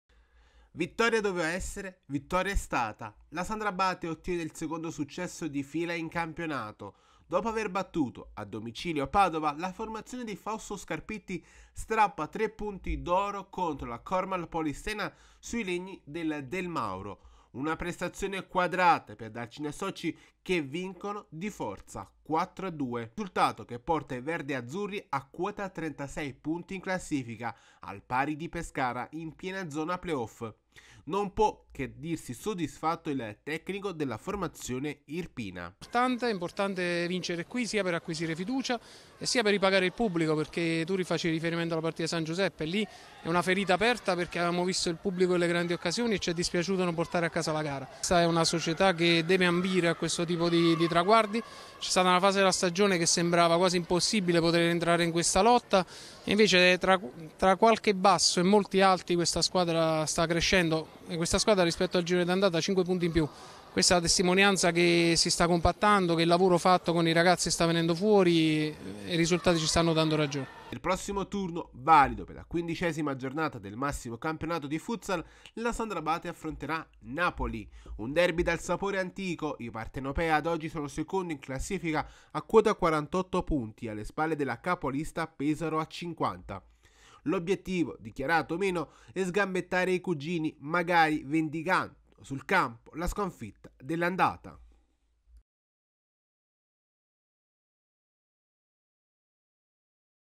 Sandro Abate, colpo playoff contro Polistena
Seconda vittoria di fila per la compagine verdeazzurra sui legni del Del Mauro. Ora si guarda già al derby contro il Napoli